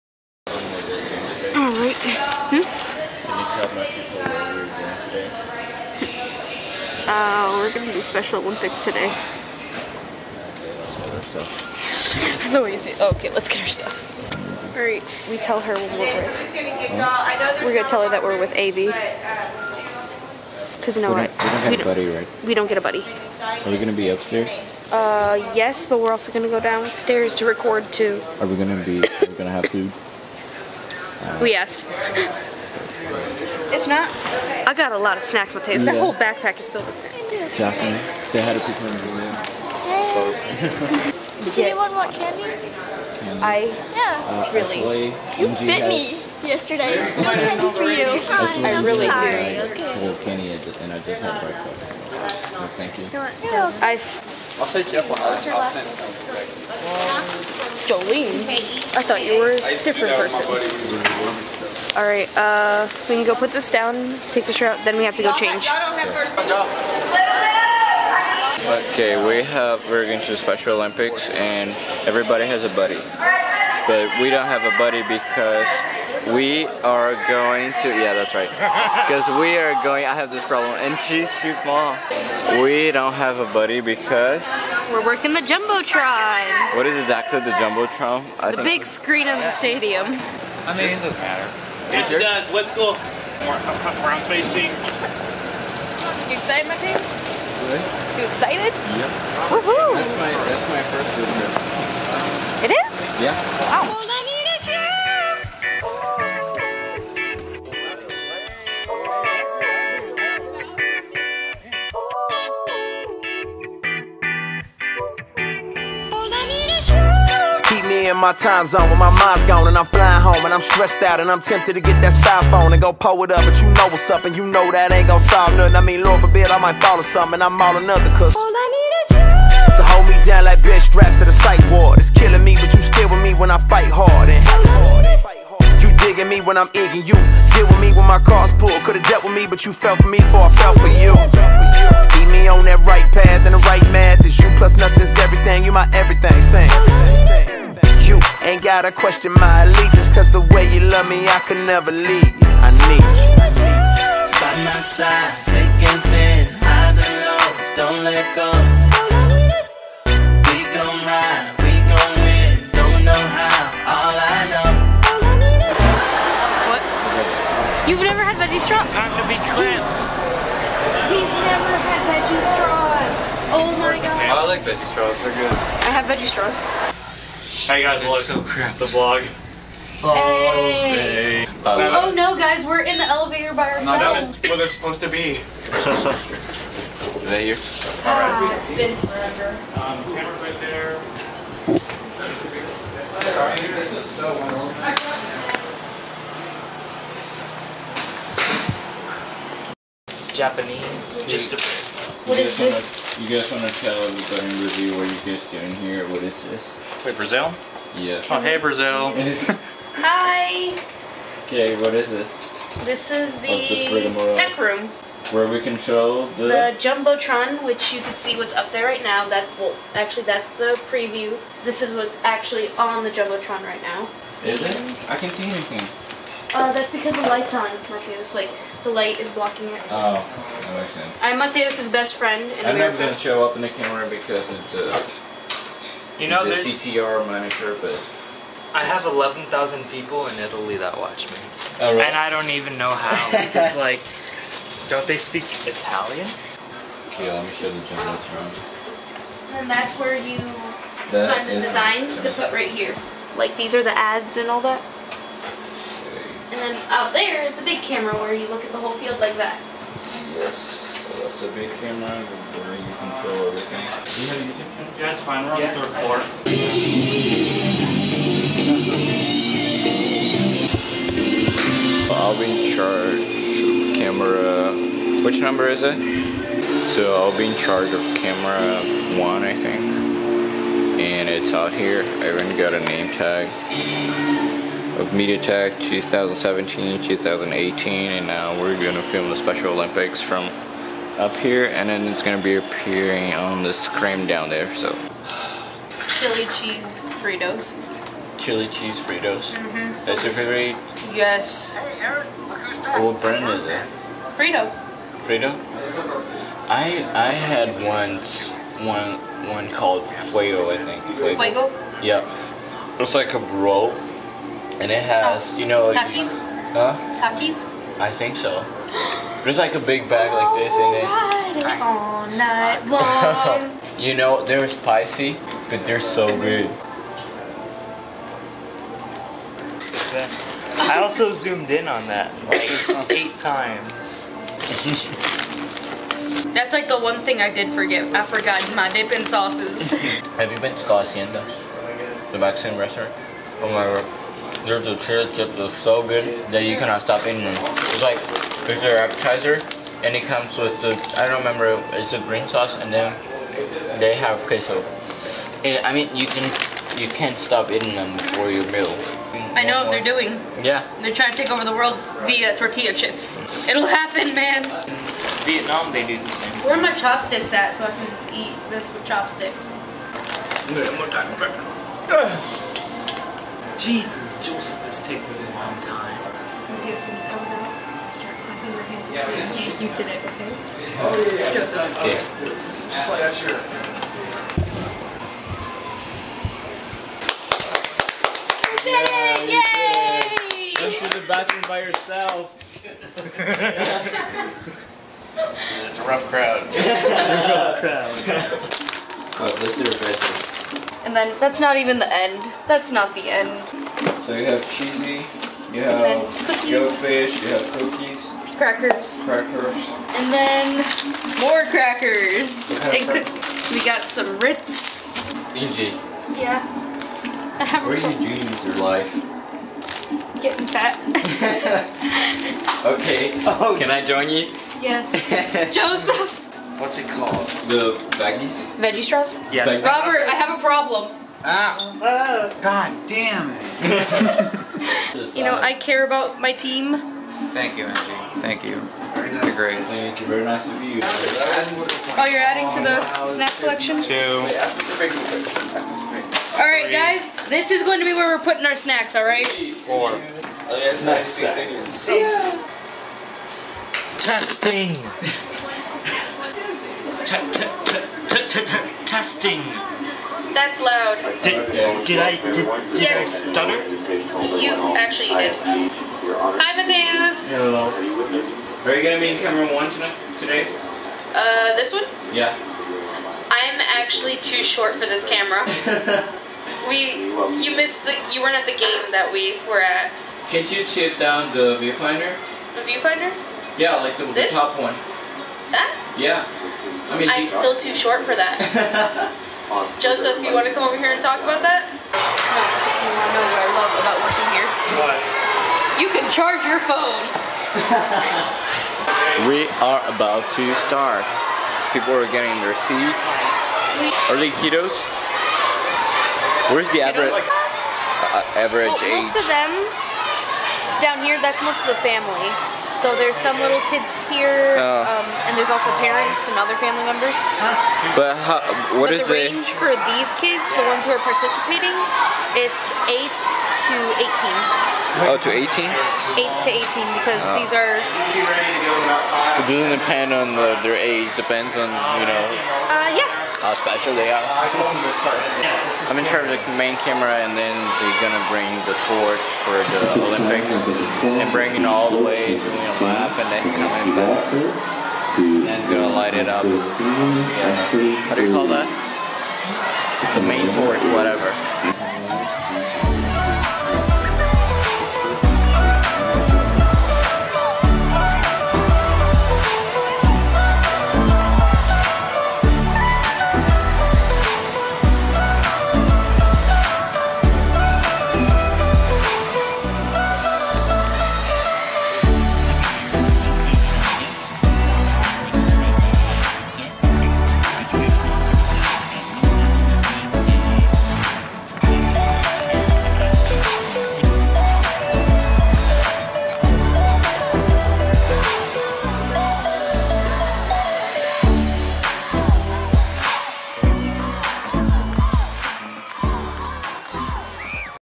FIELD TRIP PART 1 (com legenda)
My stunning first field trip as a student for Little Elm HS and as an exchange student. We spent a whole school day assisting my AV teacher to stream the competition held in our stadium in the big screen, so all the audience could see from different angles. Fun day!